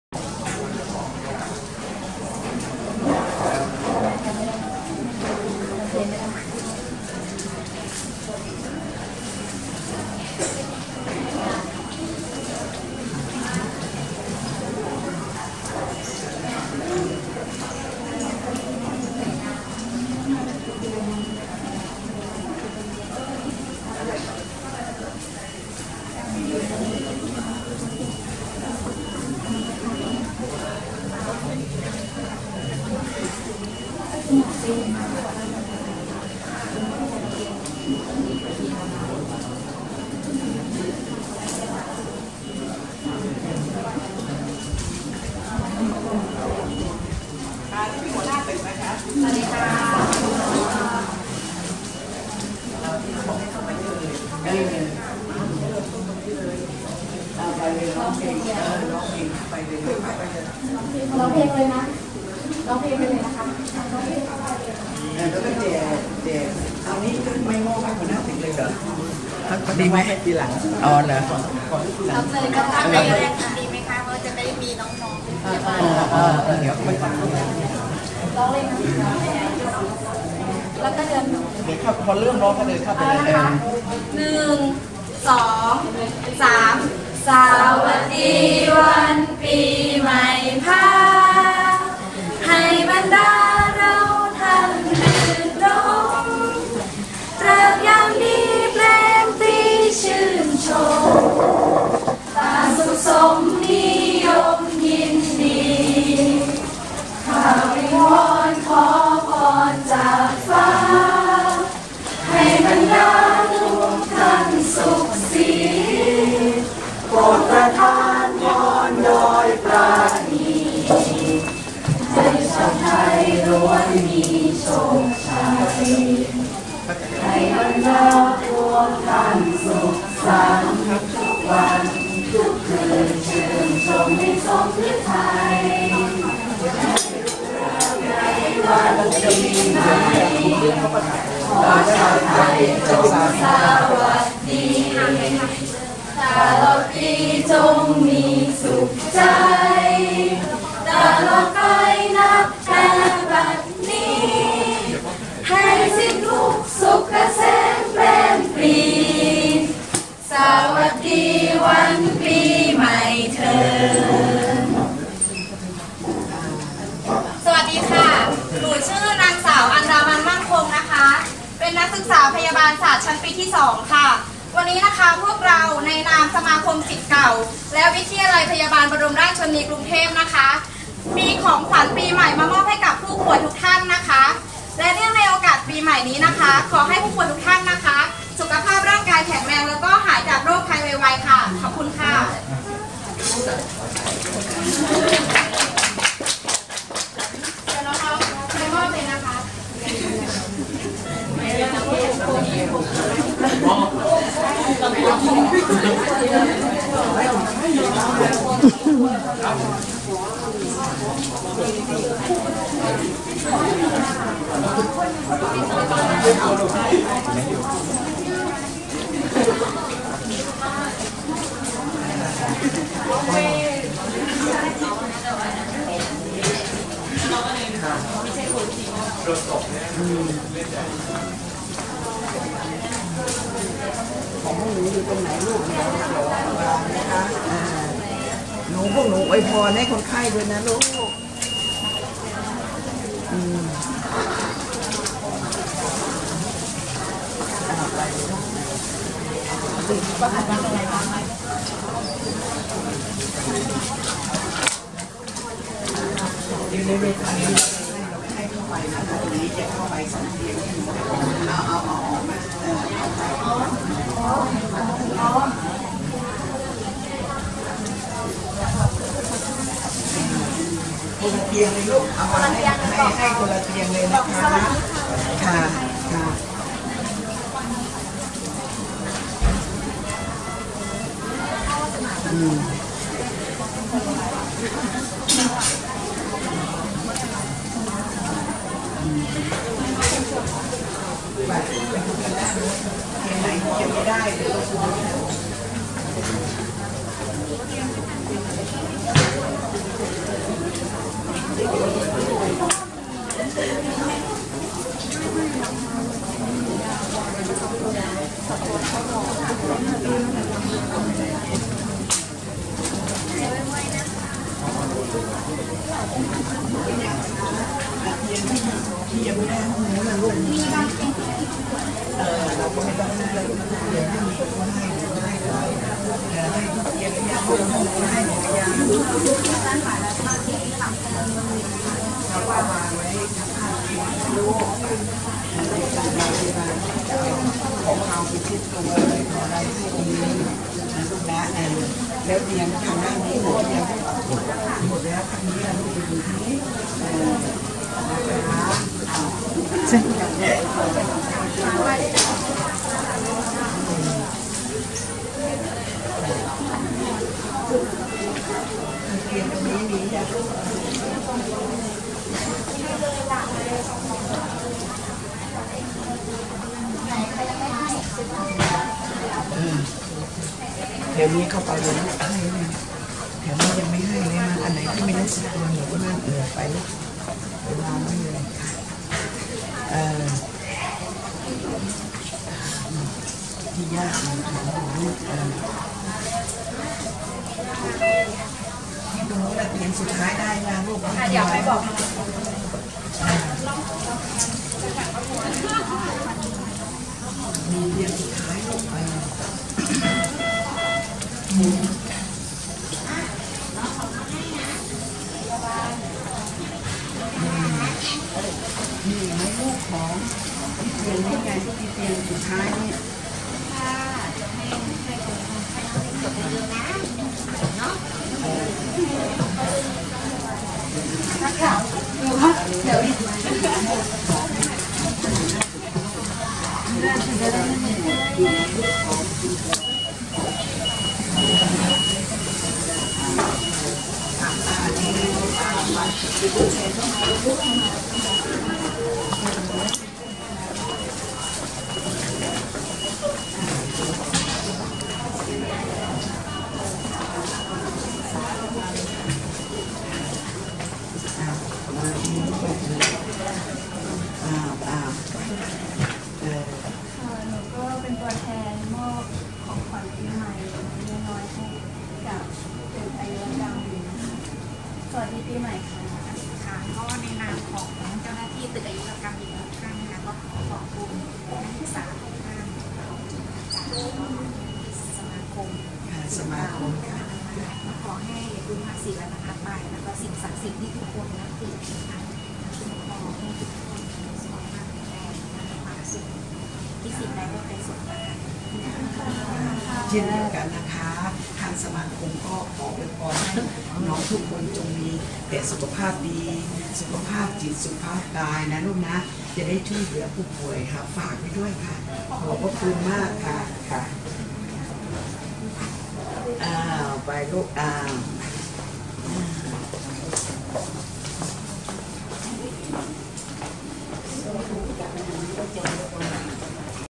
๑๐.นักเรียนวิทยาลัยพยาบาลบรมราชชนนี กรุงเทพ มอบของขวัญงานอายุรกรรมหญิงชั้น๒
๒๘ ธันวาคม ๒๕๖๐ เวลา ๑๗.๐๐ - ๑๘.๐๐ น.
คุณกลัยา ธรรมคุณ นายกสมาคมศิษย์เก่าวิทยาลัยพยาบาลบรมราชชนนี กรุงเทพ
คุณผุสดี รุ่งรังษี อดีตนายกสมาคมศิษย์เก่าวิทยาลัยพยาบาลบรมราชชนนี กรุงเทพ
นำนักเรียนวิทยาลัยพยาบาลบรมราชชชนี กรุงเทพ ชั้นปีที่๑-๒ ไปมอบของขวัญให้คนไข้สามัญตึกอายุรกรรม๑๒๐ชุด